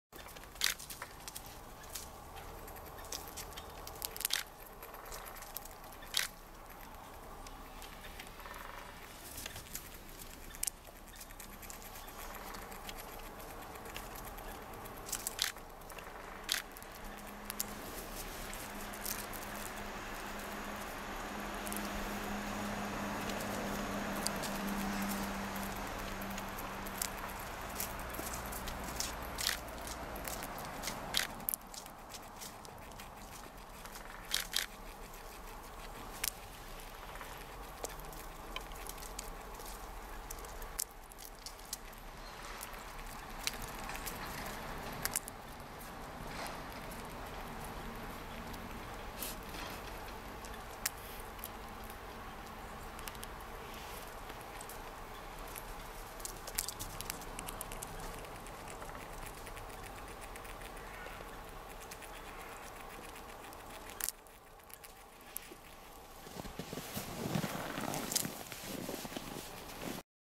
Squirrel looking for food
Squirrels are looking for food which hidden under the snow.